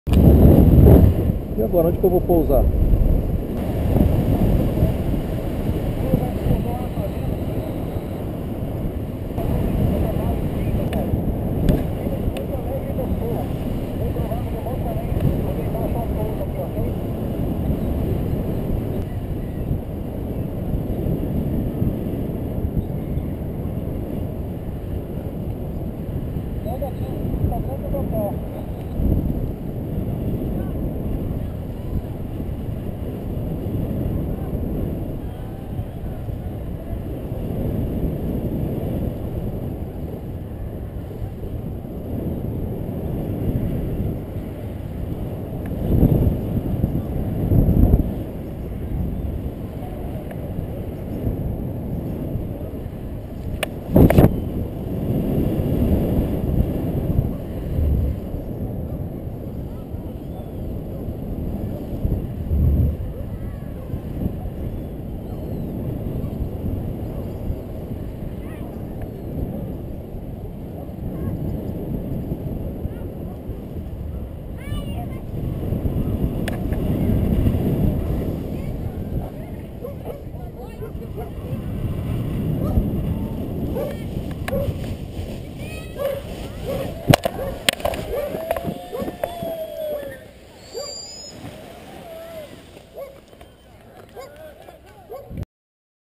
ISSO É VOO LIVRE - POUSO EM 3 PONTES - AMPARO
O BAIRRO TODO VEIO ME CUMPRIMENTAR !!! ATÉ QUE CAÍ BEM ... PULEI DE LOCAL DESCONHECIDO .